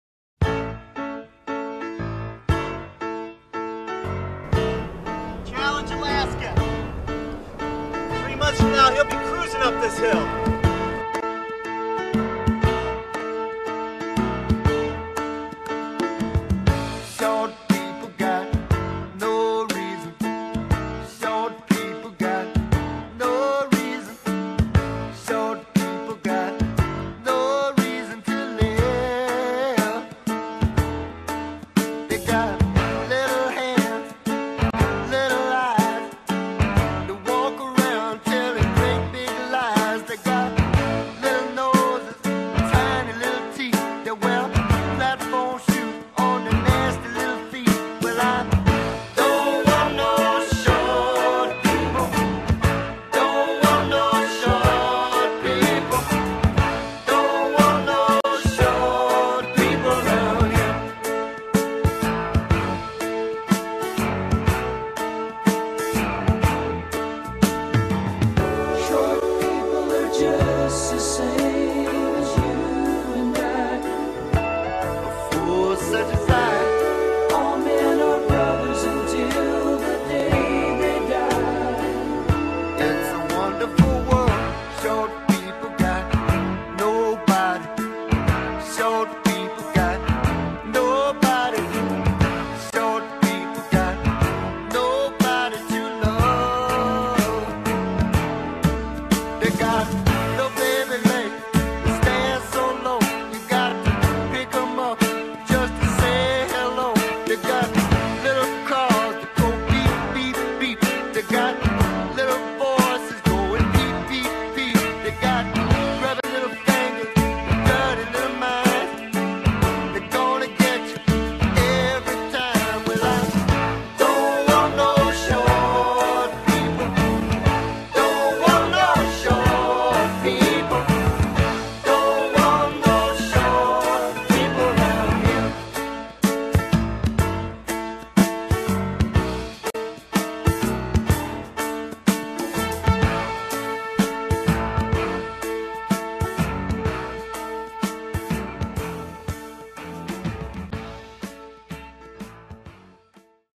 Challenge Alaska - Pitch To Randy Newman
A Pitch Piece developed for Randy Newman to participate in Challenge Alaska